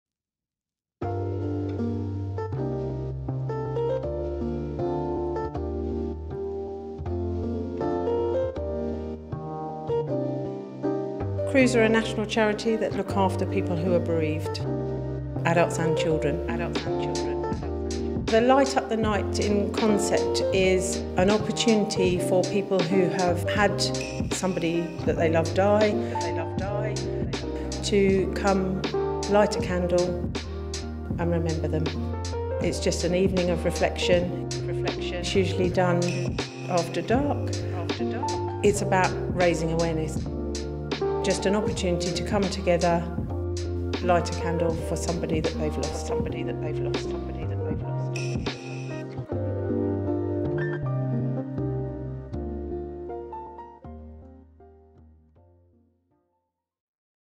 Light up the night with Cruse Bereavement
Back in November, the Cavendish Homecare Professionals team joined Cruse Bereavement Support for a candlelit event in memory of a loved one. The event, Light up the Night, took place outside the Royal Exchange Building along with a beautiful display of hundreds of tealights lit up on the stairs.

The event was open for anybody to drop by and walk, linger, light a candle and contemplate family and friends, past and present. It was a truly moving experience to see so many people pass by to remember and celebrate the lives of their loved ones.

Cavendish Homecare understands that getting the proper support when experiencing grief is important through the care we provide, and we are proud to support Cruse Bereavement to help them continue with the amazing work that they do!

A special thank you to Cruse Bereavement for helping us organise such a special event.

Here is a short video of what took place during this special evening…